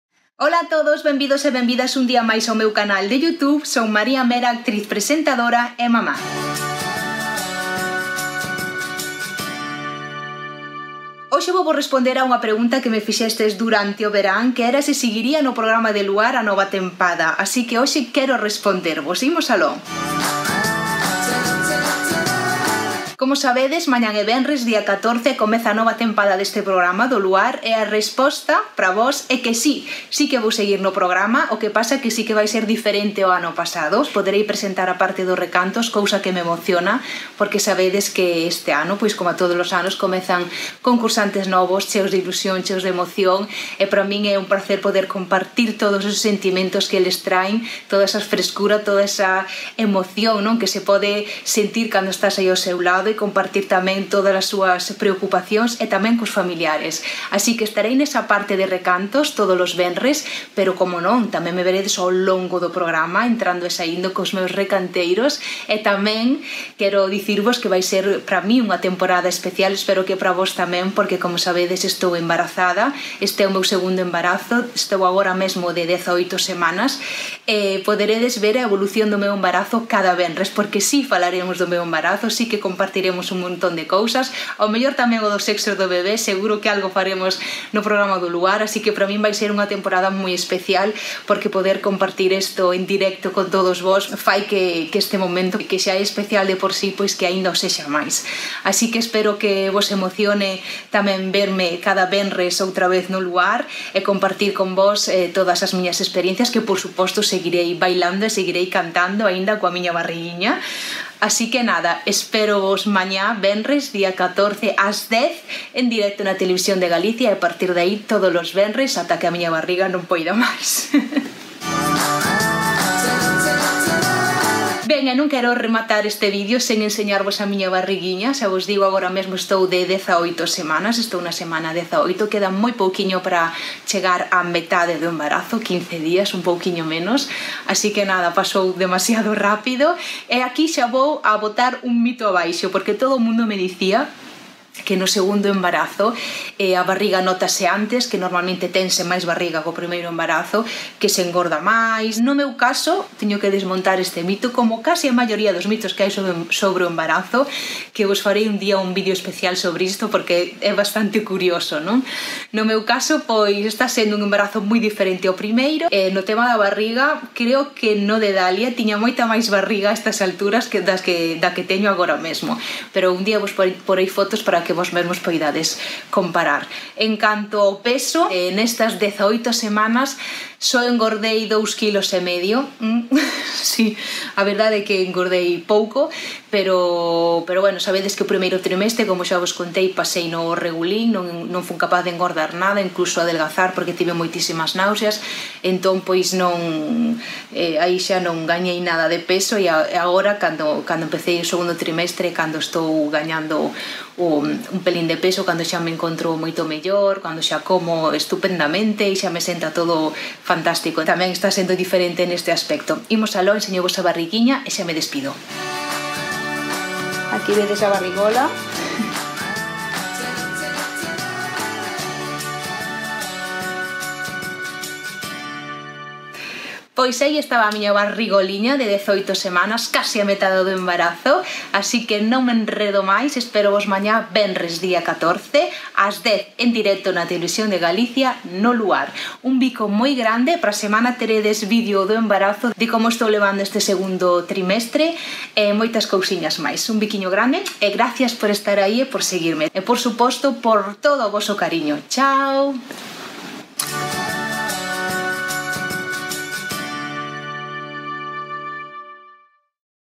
Volvo a Luar ??? + Barriga 18 Semanas
Volvo esta temporada a presentar o programa de Luar??? Aproveito o vídeo para ensinarvos como vai a miña barriga de 18 semanas ;) e canto engordei ata o momento!!